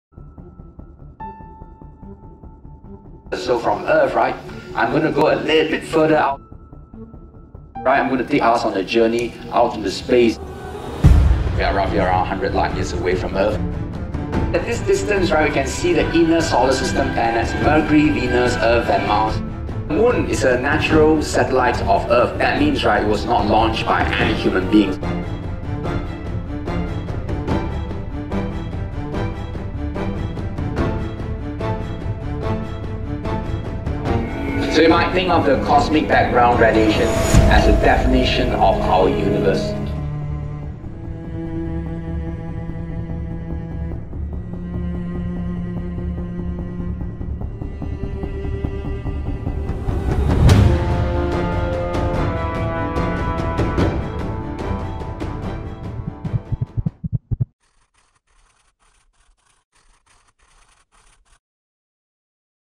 Omni-Theatre Live Planetarium Show
Come and enjoy the Omni-Theatre Live Planetarium Show where a Science Educator from Science Centre Singapore will bring you on a journey across the universe, for a larger-than-life experience!